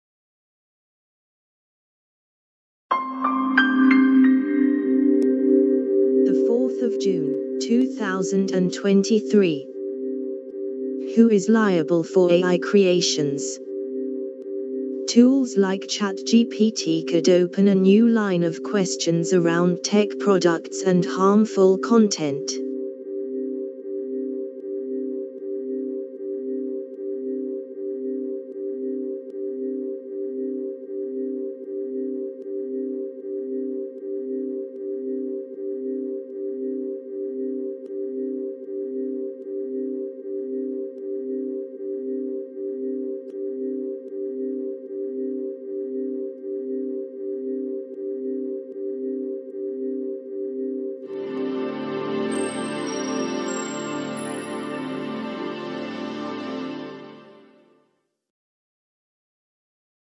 [Latest]Who Is Liable for A.I. Creations?
[Contents]
Tools like ChatGPT could open a new line of questions around tech products and harmful content.